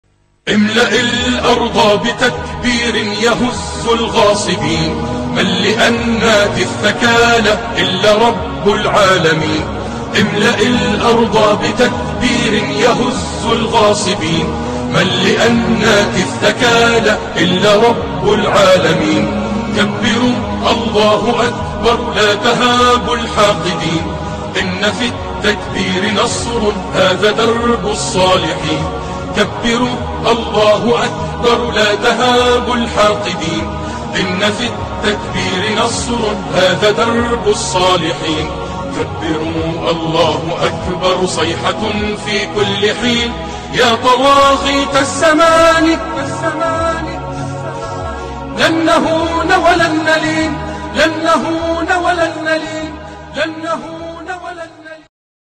Nezim ef. Halilović Muderis | ARBiH edit
Inb4 attacking me, yes I know Bosnians aren't Arabs nor do they speak Arabic. I just used this nasheed or anthem because I like it. I don't understand what he is singing.